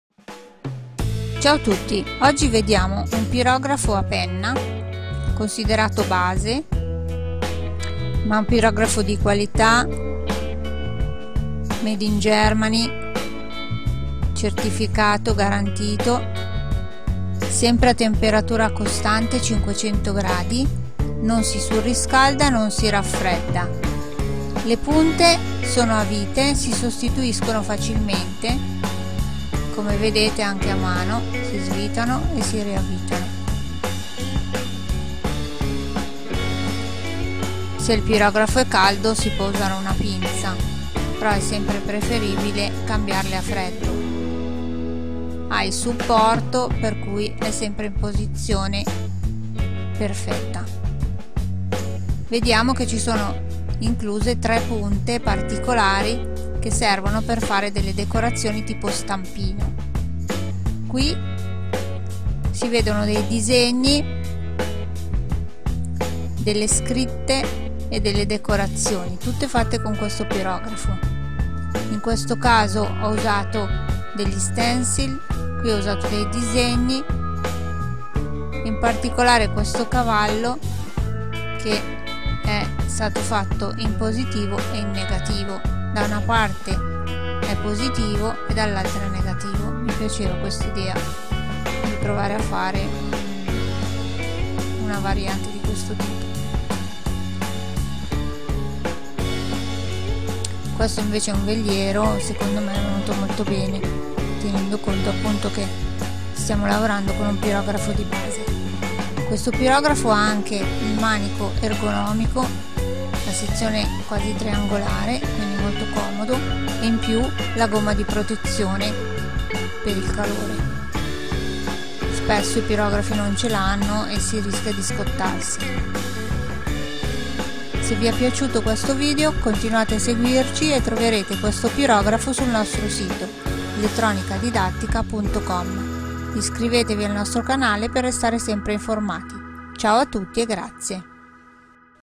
Pirografo base di qualità e funzionalità. Impariamo a pirografare con un buon pirografo su legno
Vi presentiamo un pirografo base valido ed affidabile con 9 punte intercambiabili. La pirografia è l'arte dello scrivere con il fuoco. Idee in pirografia.